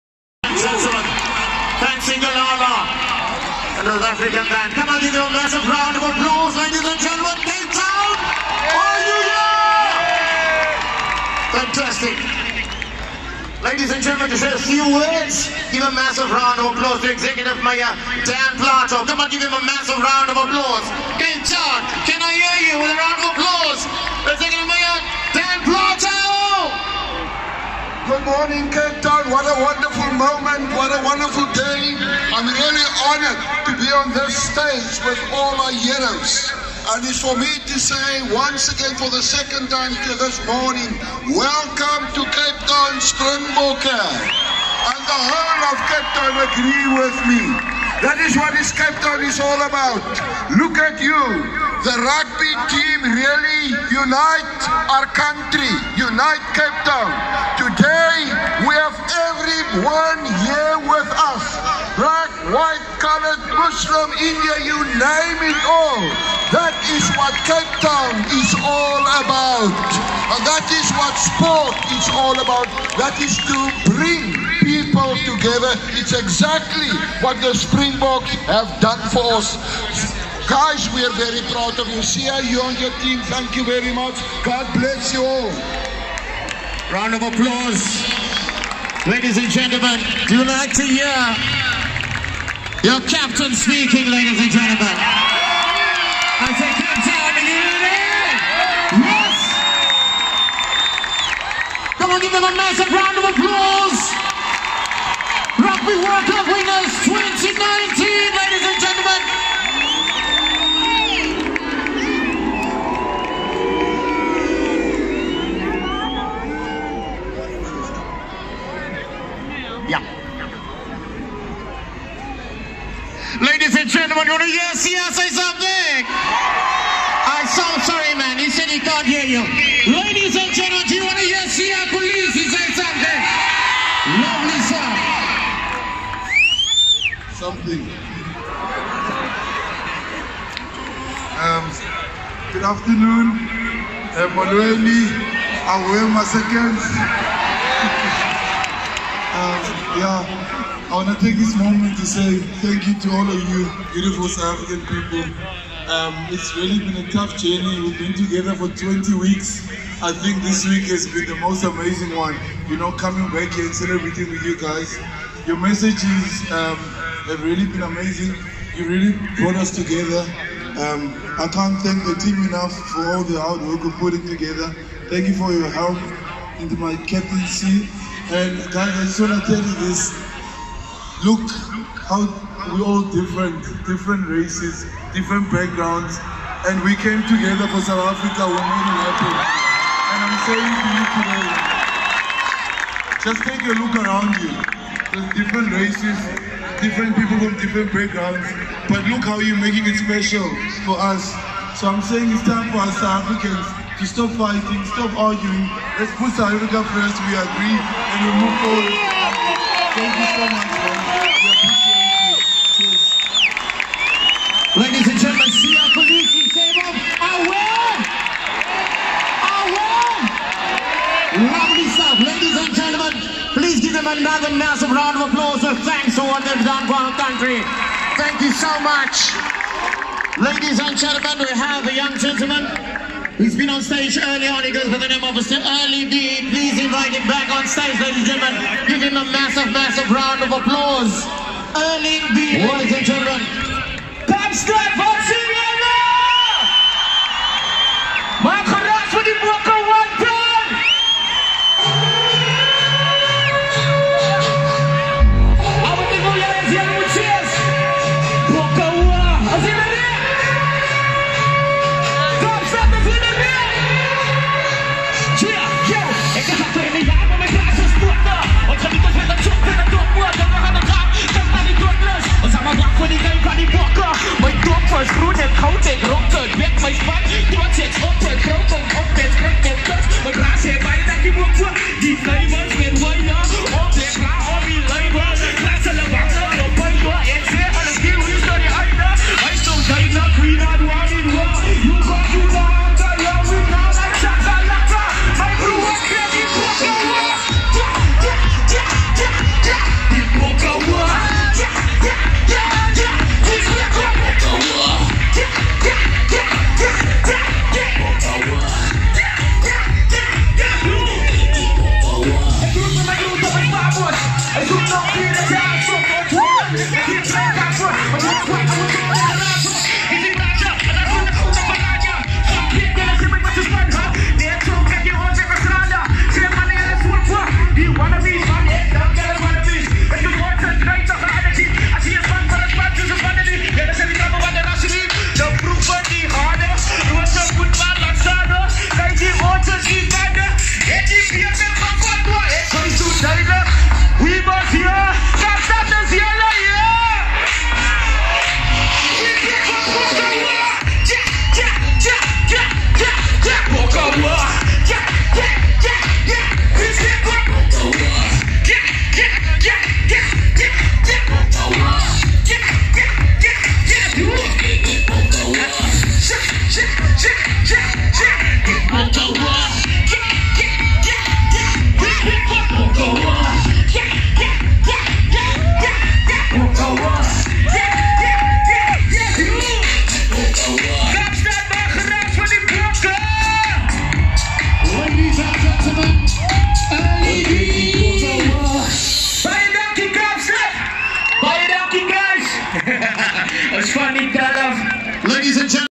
2019 WORLD RUGBY CHAMPIONSHIP WINNER SOUTH AFRICA SPRINGBOKS CAPTAIN SIYA KOLISI CELEBRATION SPEECH
The 2019 Rugby World Cup was the ninth edition of the Rugby World Cup, the quadrennial world championship for men's rugby union teams. It was hosted in Japan from 20 September to 2 November in 12 venues all across the country. The opening match was played at Tokyo Stadium in Chōfu, Tokyo with the final match being held at International Stadium Yokohama in Yokohama. This was the first time that the tournament had taken place in Asia and outside the traditional Tier 1 rugby nations.

South Africa beat England 32−12 in the final to claim their third title, equalling New Zealand's record. South Africa also became the first team to win the title after losing a match in the pool stage. The defending champions, New Zealand, finished third after defeating Wales in the bronze final.